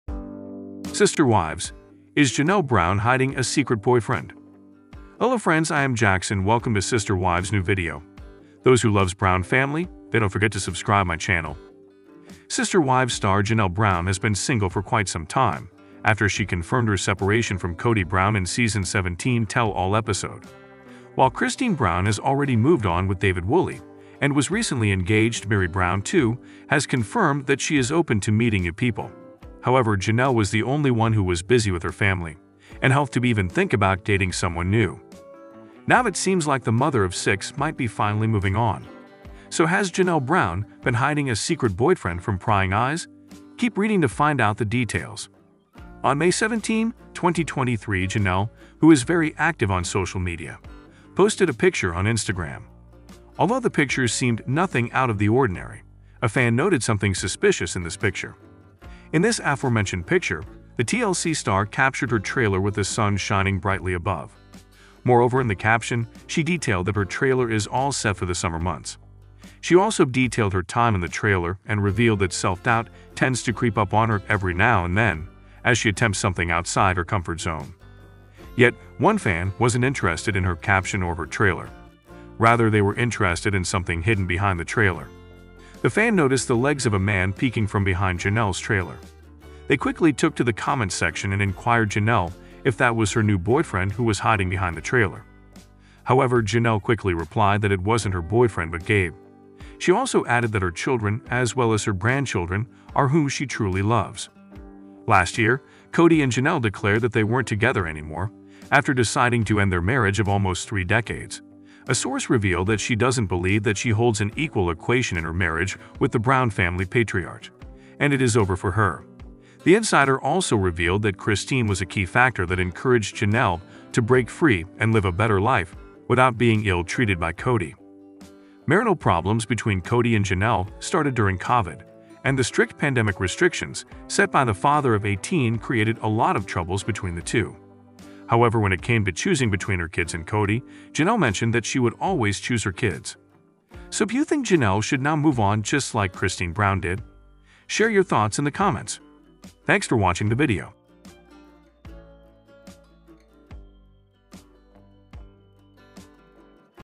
New Life Partner!! Janelle Brown Hiding A Secret Boyfriend | Kody Brown Hang
Sister Wives Is Janelle Brown Hiding A Secret Boyfriend

"Sister Wives Show" is a channel where you will find all about trending celebrity gossip. We try our best to give our audience the most truthful Gossip.

⚪Sister Wives Show